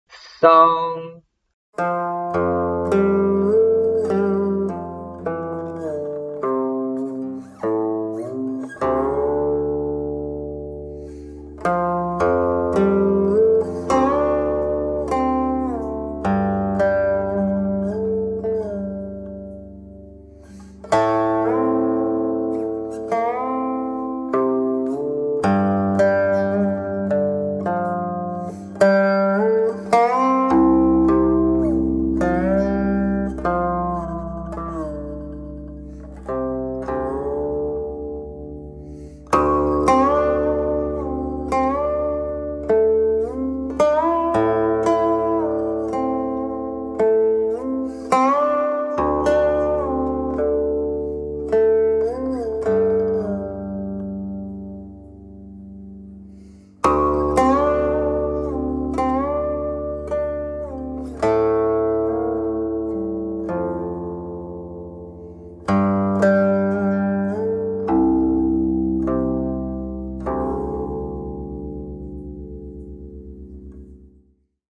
Calligraphie chinoise un caractère par jour n°431 : 桑 mûrier, mulberry
Calligraphie chinoise niveau débutant n°431 - 桑 mûrier, mulberry

Le caractère 桑 est un pictogramme, figure d'un mûrier avec ses feuilles.